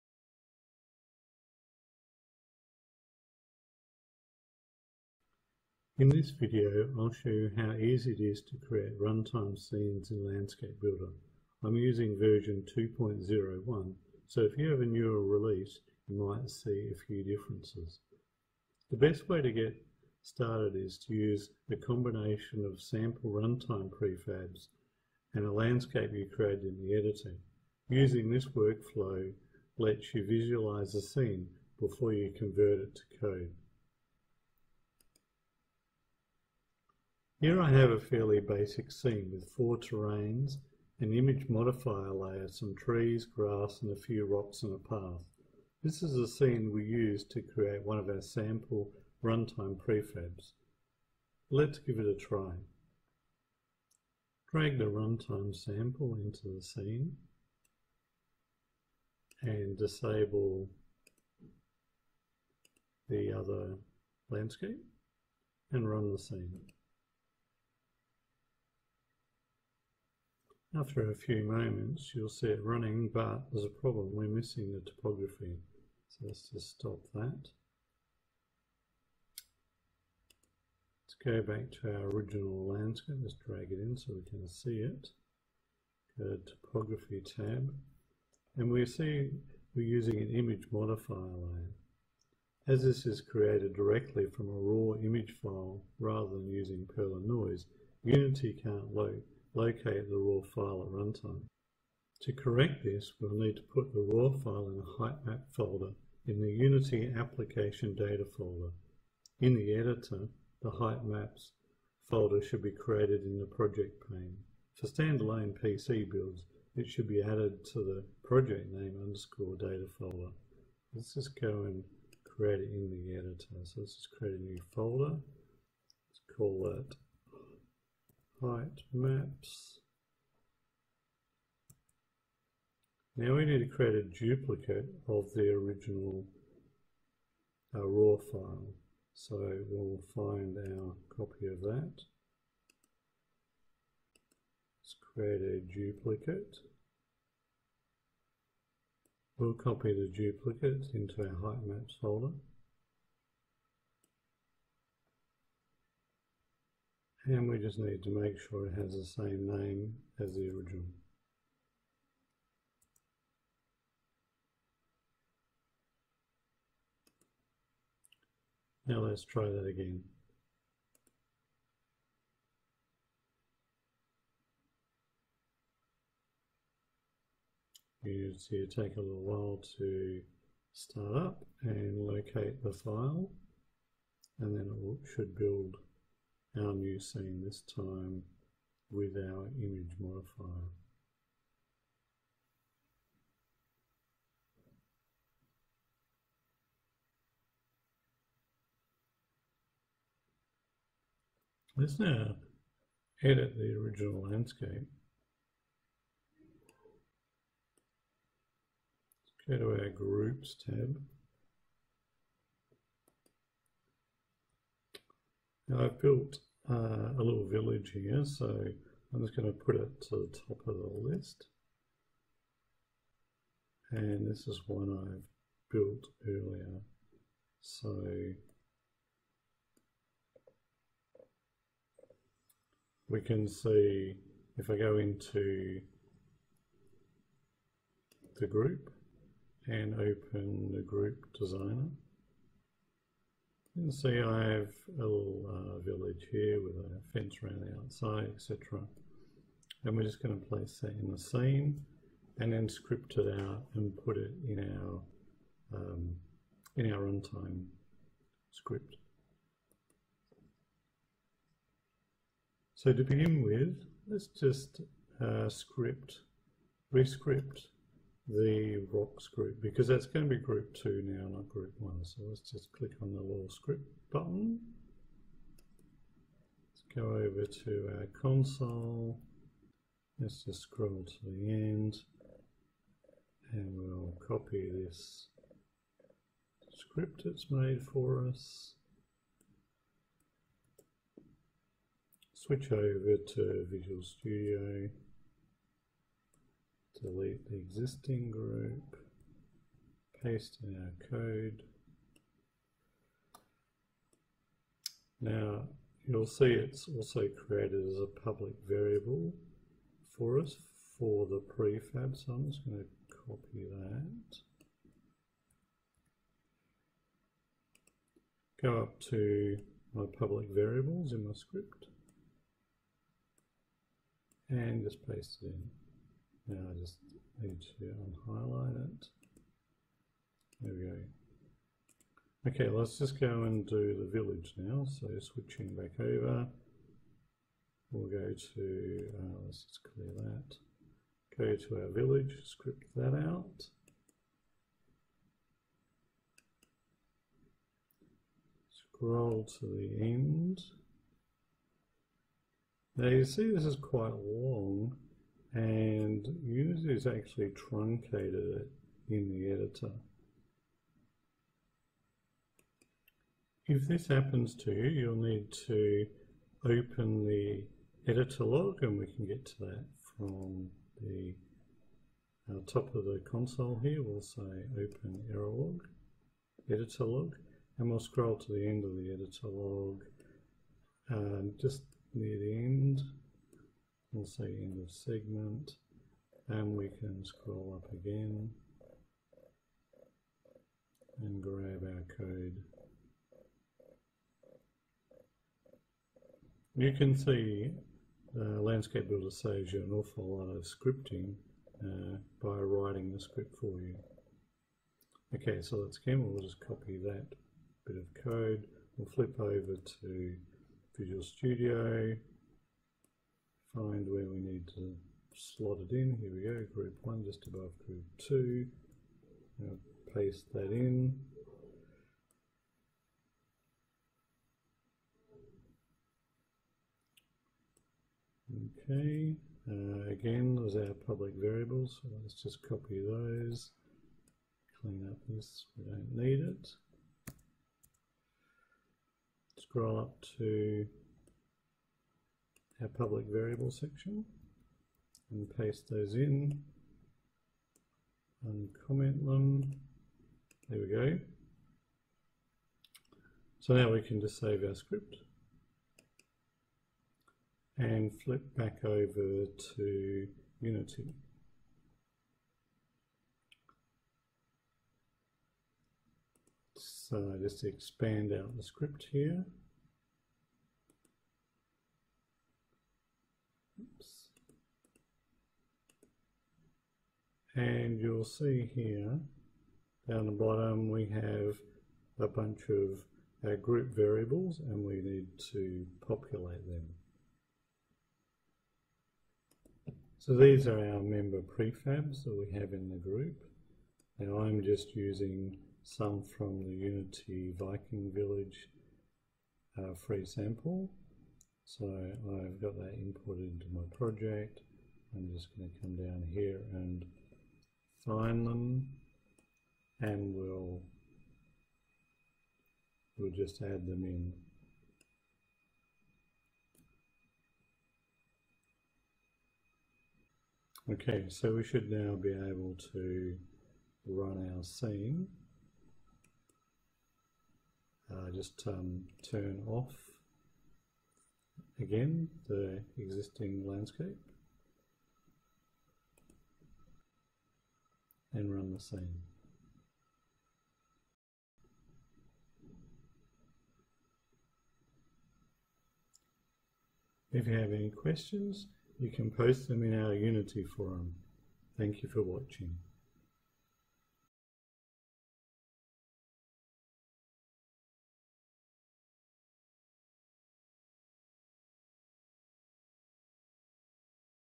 Landscape Builder 2 Runtime tutorial
This is a simple tutorial that describes how to create runtime builds using Landscape Builder 2.0 for Unity.